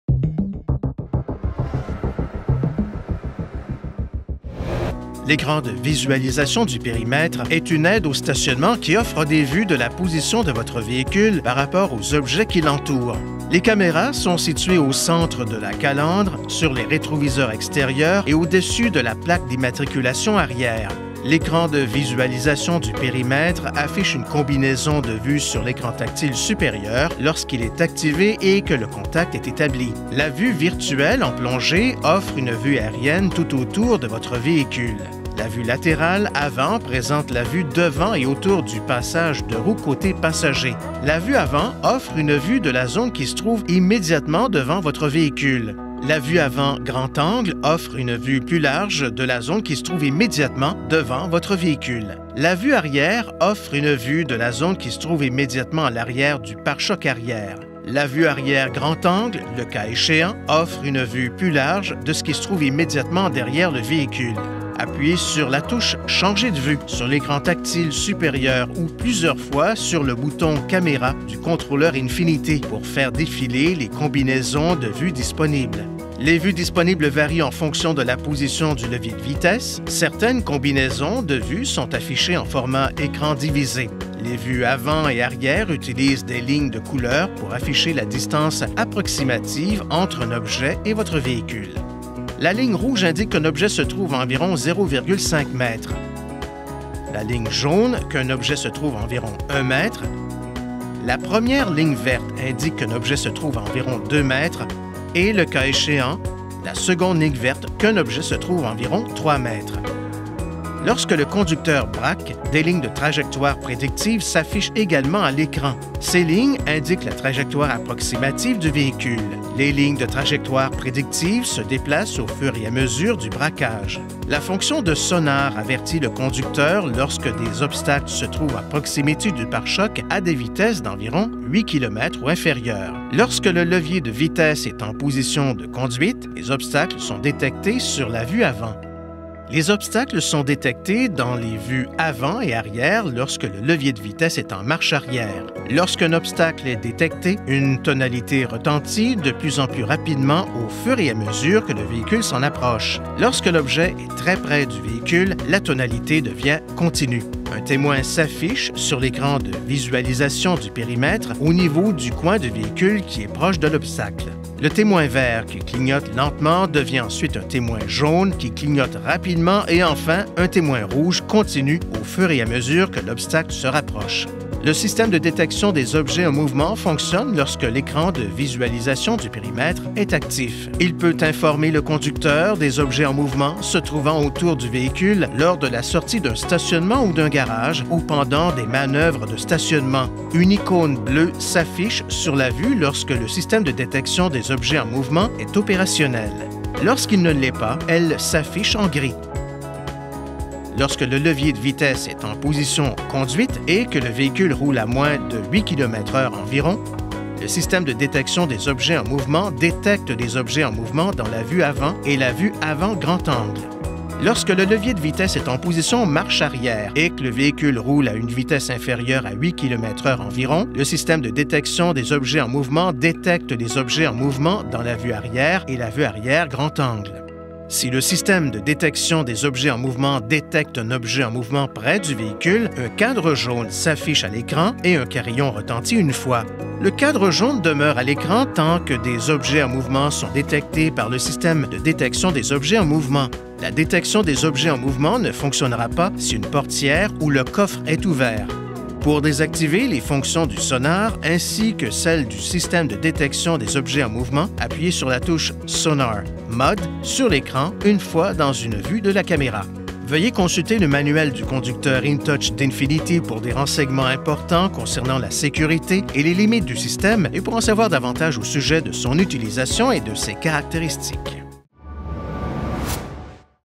INFINITI - Écran de visualisation du périmètreᴹᴰ avec détection des objets en mouvement
L’écran de visualisation du périmètre est une aide au stationnement qui offre des vues de la position de votre véhicule par rapport aux objets qui l’entourent. Les caméras sont situées au centre de la calandre, sur les rétroviseurs extérieurs et au-dessus de la plaque d’immatriculation arrière. L’écran de visualisation du périmètre affiche une combinaison de vues sur l’écran tactile supérieur lorsqu’il est activé et que le contact est établi. La vue virtuelle en plongée offre une vue aérienne tout autour de votre véhicule. La vue latérale avant présente la vue devant et autour du passage de roue côté passager. La vue avant offre une vue de la zone qui se trouve immédiatement devant votre véhicule. La vue avant grand-angle offre une vue plus large de la zone qui se trouve immédiatement devant votre véhicule. La vue arrière offre une vue de la zone qui se trouve immédiatement à l’arrière du pare-chocs arrière. La vue arrière grand-angle, le cas échéant, offre une vue plus large de ce qui se trouve immédiatement derrière le véhicule. Appuyez sur la touche « Changer de vue » sur l’écran tactile supérieur ou plusieurs fois sur le bouton CAMÉRA du contrôleur INFINITI pour faire défiler les combinaisons de vues disponibles. Les vues disponibles varient en fonction de la position du levier de vitesses. Certaines combinaisons de vues sont affichées en format écran divisé. Les vues avant et arrière utilisent des lignes de couleur pour afficher la distance approximative entre un objet et votre véhicule. La ligne rouge indique qu’un objet se trouve à environ 0,5 m, la ligne jaune, qu’un objet se trouve à environ 1 m. La première ligne verte indique qu’un objet se trouve à environ 2 m et, le cas échéant, la seconde ligne verte, qu’un objet se trouve à environ 3 m. Lorsque le conducteur braque, des lignes de trajectoire prédictive s’affichent également à l’écran. Ces lignes indiquent la trajectoire approximative du véhicule. Les lignes de trajectoire prédictive se déplacent au fur et à mesure du braquage. La fonction de sonar avertit le conducteur lorsque des obstacles se trouvent à proximité du pare-chocs à des vitesses d’environ 8 km/h ou inférieures. Lorsque le levier de vitesses est en position de conduite, les obstacles sont détectés sur la vue avant. Les obstacles sont détectés dans les vues avant et arrière lorsque le levier de vitesses est en marche arrière. Lorsqu’un obstacle est détecté, une tonalité retentit de plus en plus rapidement au fur et à mesure que le véhicule s’en approche. Lorsque l’objet est très près du véhicule, la tonalité devient continue. Un témoin s’affiche sur l’écran de visualisation du périmètre au niveau du coin du véhicule qui est proche de l’obstacle. Le témoin vert qui clignote lentement devient ensuite un témoin jaune qui clignote rapidement, et enfin un témoin rouge continu au fur et à mesure que l’obstacle se rapproche. Le système de détection des objets en mouvement fonctionne lorsque l’écran de visualisation du périmètre est actif. Il peut informer le conducteur des objets en mouvement se trouvant autour du véhicule lors de la sortie d’un stationnement ou d’un garage, ou pendant des manœuvres de stationnement. Une icône bleue s’affiche sur la vue lorsque le système de détection des objets en mouvement est opérationnel. Lorsqu’il ne l’est pas, elle s’affiche en gris. Lorsque le levier de vitesses est en position Conduite et que le véhicule roule à moins de 8 km/h environ, le système de détection des objets en mouvement détecte les objets en mouvement dans la vue avant et la vue avant grand-angle. Lorsque le levier de vitesses est en position Marche arrière et que le véhicule roule à une vitesse inférieure à 8 km/h environ, le système de détection des objets en mouvement détecte les objets en mouvement dans la vue arrière et la vue arrière grand-angle. Si le système de détection des objets en mouvement détecte un objet en mouvement près du véhicule, un cadre jaune s’affiche à l’écran, et un carillon retentit une fois. Le cadre jaune demeure à l’écran tant que des objets en mouvement sont détectés par le système de détection des objets en mouvement. La détection des objets en mouvement ne fonctionnera pas si une portière ou le coffre est ouvert. Pour désactiver les fonctions du sonar, ainsi que celles du système de détection des objets en mouvement, appuyez sur la touche « Sonar MOD » sur l’écran, une fois dans une vue de la caméra.

Veuillez consulter le manuel du conducteur InTouchMC d’INFINITI pour des renseignements importants concernant la sécurité et les limites du système, et pour en savoir davantage au sujet de son utilisation et de ses caractéristiques.